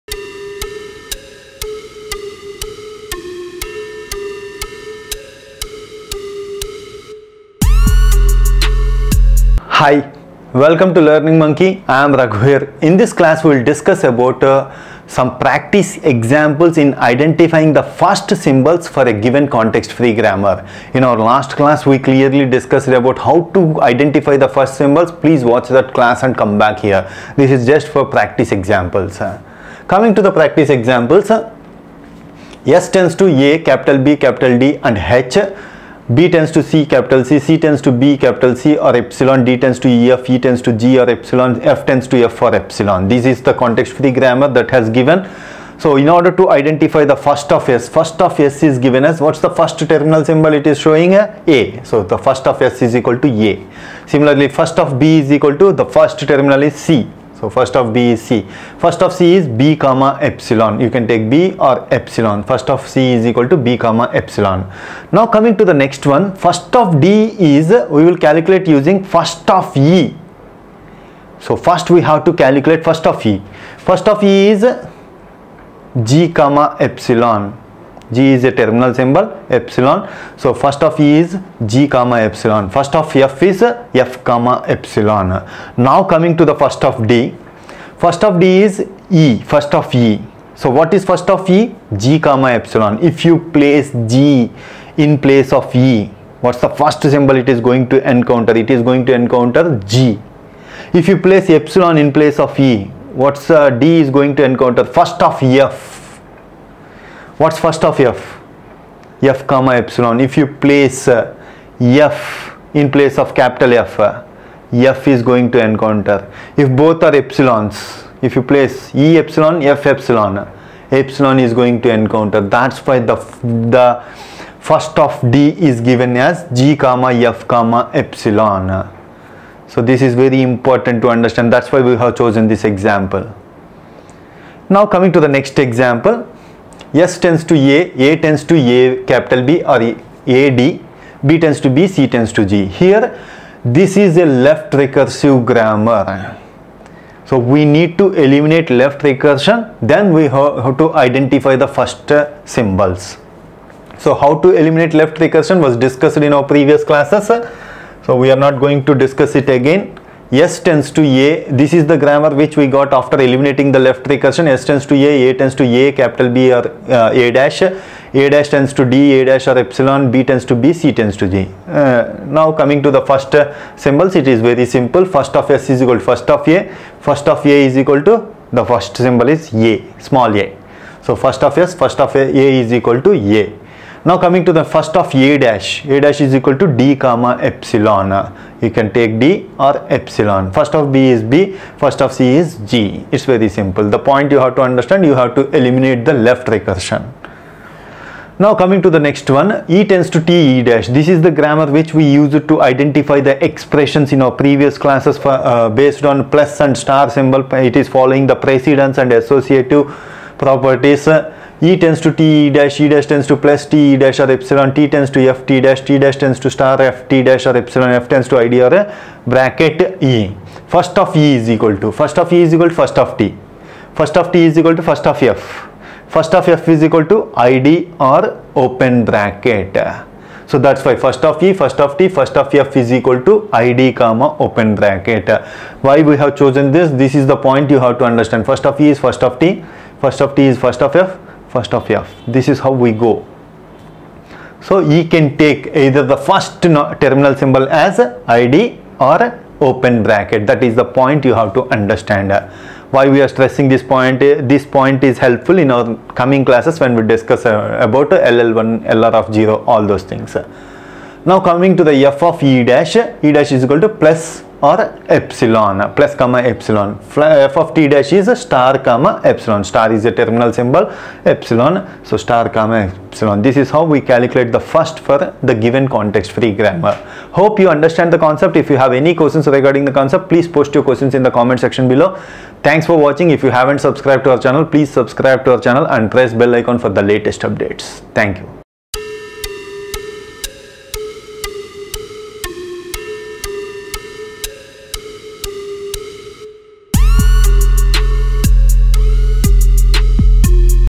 First Examples in Compiler || Lesson 19 || Compiler Design || Learning Monkey
First Examples in Compiler
In this class, We discuss the First Examples in Compiler.
The reader should have prior knowledge of How to find the first symbols. Click Here.
Example 1:
S – aBDh
B – cC
C – bC | ε
D – EF
E – g | ε
F – f | ε
Solution:
First(S) = {a}
First(B) = {c}
First(C) = {b, ε}
First(D) = First(E)
First(E) = {g, ε}
Epsilon is present in the first symbols of E. So place epsilon in place of E in the production D – EF
If we place epsilon in place of E in the production D – EF., we need to find the First(F) for First(D).
First(F) = {f, ε}
Finally, First(D) = First(E) U First(F)
We get First(D) = {g, ε} U {f, ε} = {g, f, ε}
Example 2:
S – A
A – aB | Ad
B – b
C – g
The given grammar is having left recursion.
We need to eliminate left recursion and continue finding the first symbols.
How to eliminate left recursion? Click Here.
S – A
A – aBA’
A’ – dA’ | ε
B – b
C – g
Solution:
First(S) = First(A) = a
First(A’) = {d, ε}
First(B) = {b}
First(C) = {g}
Example 3:
E – TE’
E’ – +TE’ | ε
T – FT’
T’ – *FT’ | ε
F – id | (E)
Solution:
First(E) = First(T) = First(F) = {id, (}
First(E’) = {+, ε}
First(T’) = {*, ε}